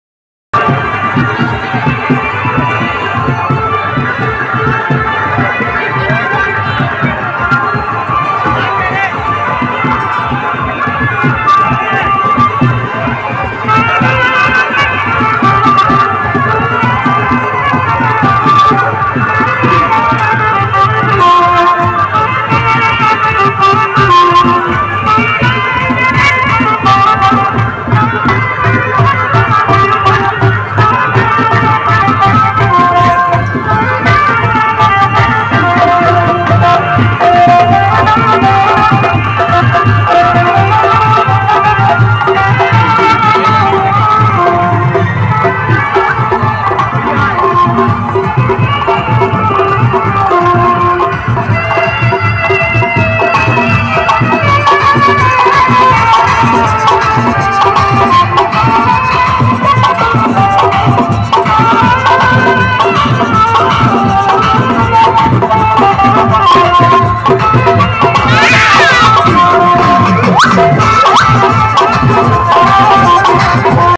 Hazaribagh Muharram julus Mandai kalan 🔥🔥 Full atank Hussaini Group 2022
hazaribagh International Muharram procession hard roll Mandai kalan 🔥🔥🔥 2022 ka sbse jyada abadi ka julus Mandai kalan ka